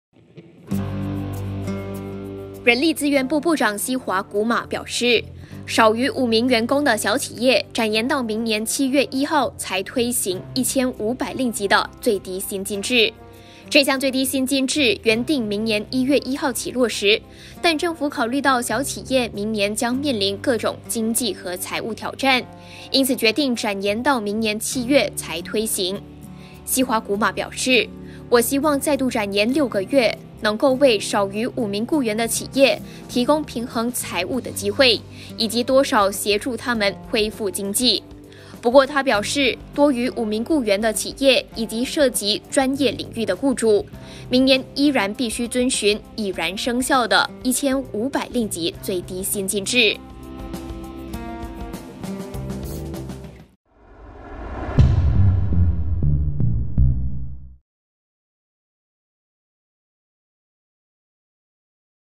最低薪制全面落实再延 小企业可明年7月才实行
人力资源部部长西华古玛表示，少于5名员工的小企业，展延到明年7月1日才推行1500令吉的最低薪金制。

这项最低薪金制原定明年1月1日起落实，但政府考虑到小企业明年将面临各种经济和财务挑战，因此决定展延到明年7月才推行。

西华古玛表示，“我希望再度展延六个月，能够为少于五名雇员的企业，提供平衡财务的机会，以及多少协助他们恢复经济。”

不过，他表示，多于五名雇员的企业及涉及专业领域的雇主，明年依然必须遵循已经生效的1500令吉最低薪资制。